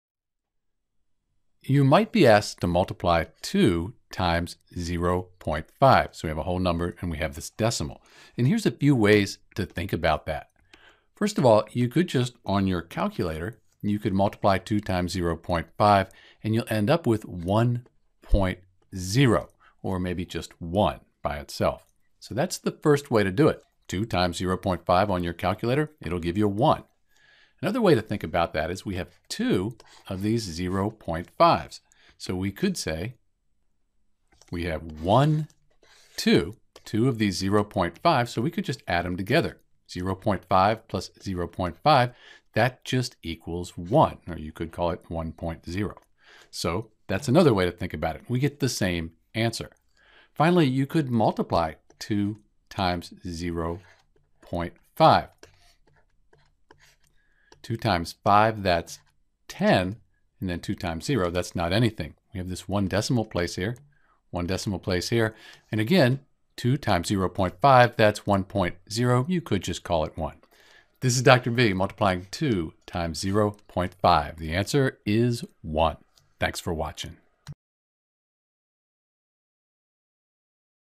How to Multiply 2 times 0.5 ( 2 x 0.5)
In this video well multiple the whole number 2 times the decimal 0.5.  You can do this on a calculator and you will get 1.

You can also think of it as 0.5 + 0.5 since 2 x  0.5 means we have 0.5 two times.  You get 1.

Finally you could multiple longhand  and you will end up with 1 as your answer.